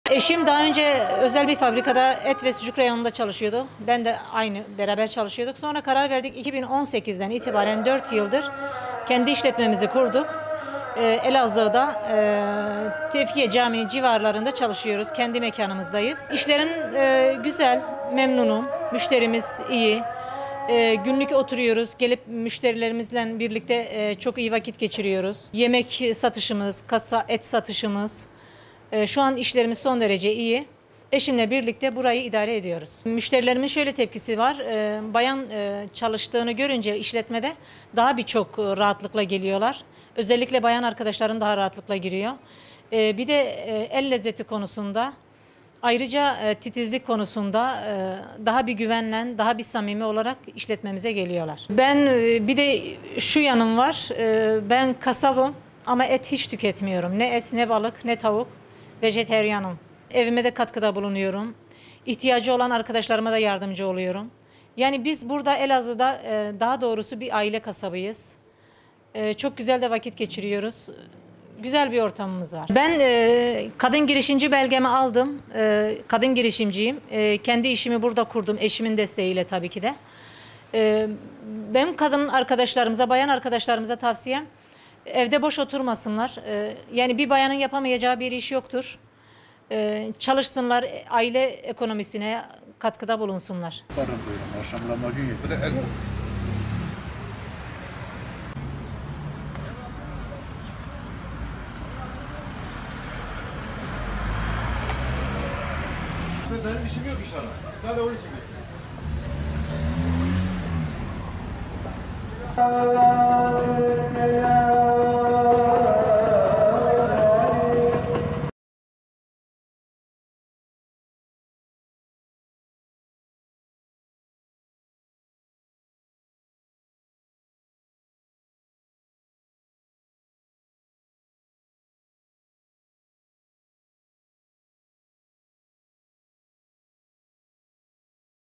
Vejetaryen Olması Kasap Olmasına Engel Olmadı!
Kasap kocasının 30 yıllık mesleğine ortak olan Elazığ’ın ilk kadın kasabı Mürüvvet Burhan, vejetaryen olmasıyla dikkat çekiyor.

Elazığ'da 3 çocuk annesi Mürüvvet Burhan (46), erkek mesleği olarak bilinen kasaplığı yapmaya başladı. Mürüvvet Burhan, yaklaşık 25 yıl bir fabrikada kasap reyonunda çalışan eşiyle birlikte 4 yıl önce açtığı kasap dükkanını çalıştırıyor. Kasap kadını karşılarında gören müşteriler başlangıçta şaşırırken, işinin ehli olduğunu anlayınca kendisini takdir ediyor. Kasap dükkanını işleterek aile bütçesine katkı sağlayan kadın girişimci Burhan; kırmızı et, tavuk ve balık gibi ürünleri tüketmeme özelliğiyle de dikkat çekiyor.

Eşinin desteğiyle kasap dükkanı açtıklarını belirten Burhan, "Eşimle birlikte daha önce bir fabrikada et ve sucuk reyonunda çalışıyorduk. Daha sonra karar verdik ve 2018 yılında kendi işletmemizi kurduk. Elazığ’da Tevfikiye Camii civarında çalışıyoruz. İşlerim güzel, memnunum. Müşterilerimizle beraber çok güzel vakit geçiriyoruz. Yemek ve et satışlarımız iyi. Eşimle birlikte burayı idare ediyoruz”